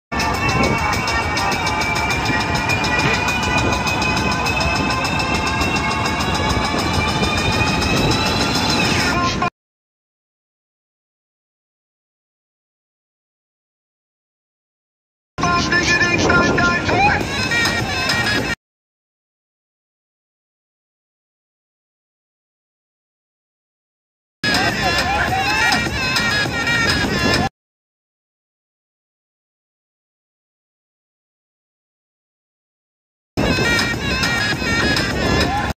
Sony Xperia XZs Superslow Motion with Jbl Flip 4.
Awwweeesssommeeee. 😍 😍 😍 😍 😍 😍

Jbl Flip 4 full water proof with Xperia XZs slow mo.